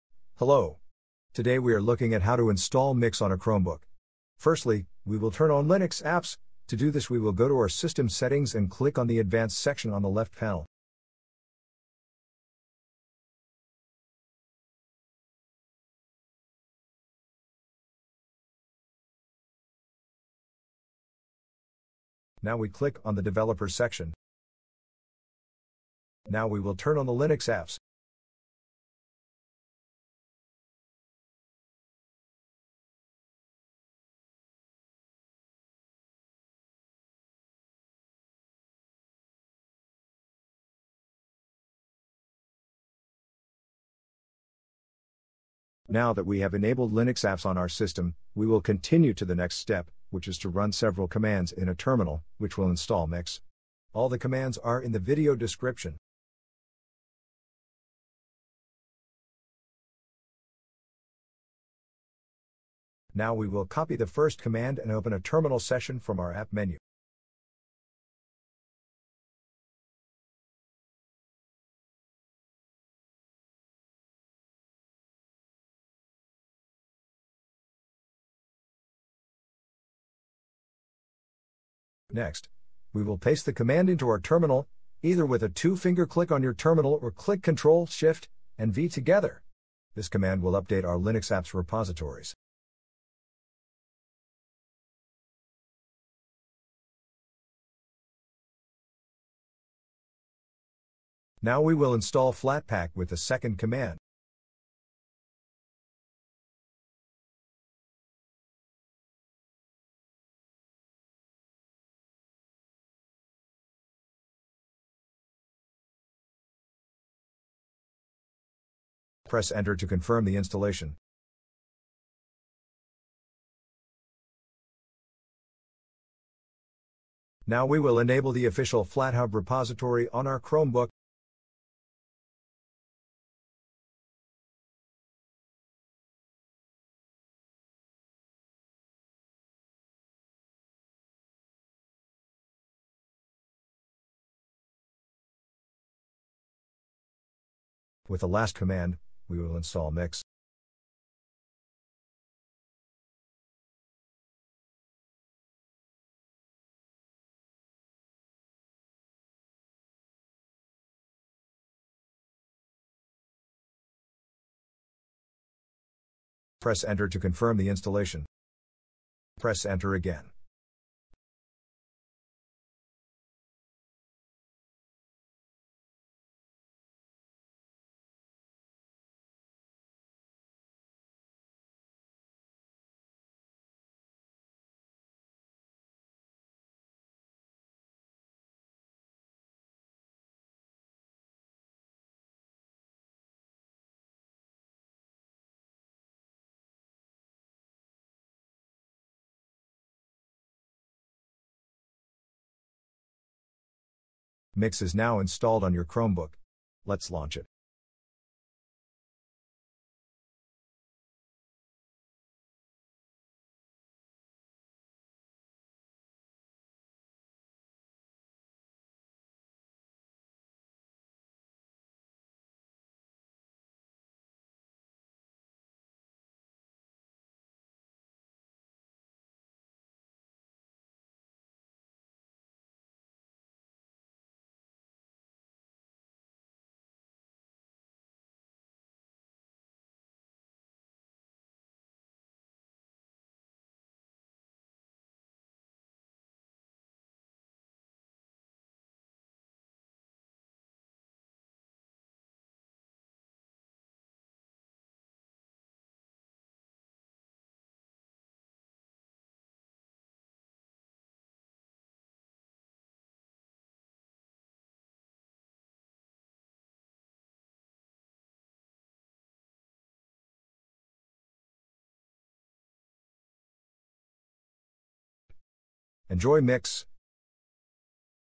How to install Mixxx on a Chromebook
In this video, we are looking at how to install Mixxx on a Chromebook.